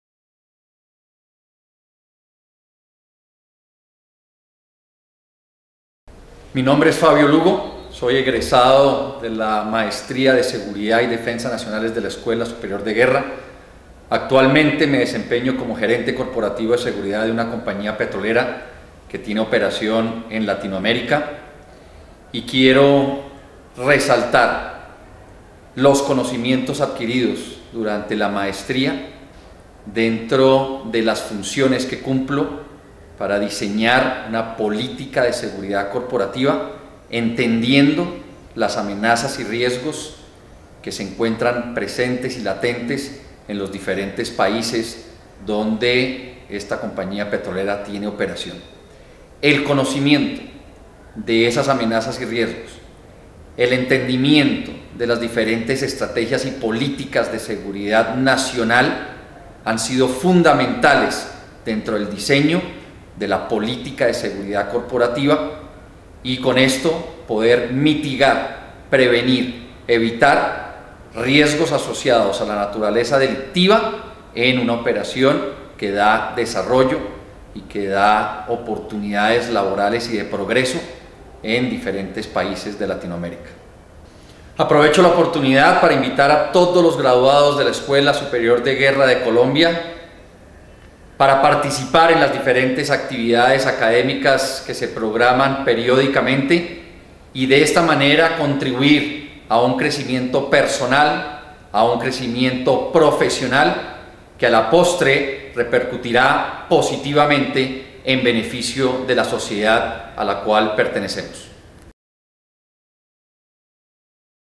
Fabio Lugo
Fabio Lugo, es GraduadoESDEG de la Maestría en Seguridad y Defensa Nacionales y nos cuenta su experiencia en el siguiente video.